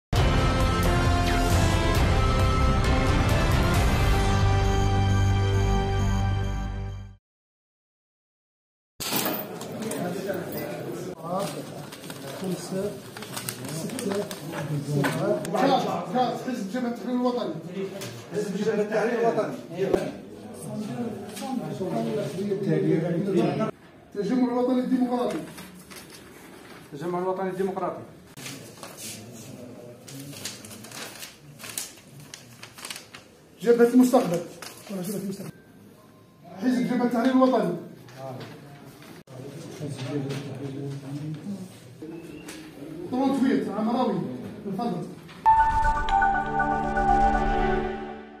محليات 27 نوفمبر: انطلاق عملية فرز اصوات الناخبين في مكاتب الاقتراع بالجلفة
الجلفة (وأج)- انطلقت عقب غلق مكاتب الاقتراع بولاية الجلفة ، عملية فرز اصوات الناخبين المعبر عنها في اطار الانتخابات المحلية لتجديد المجالس الشعبية البلدية و الولائية لـ 27 نوفمبر الجاري.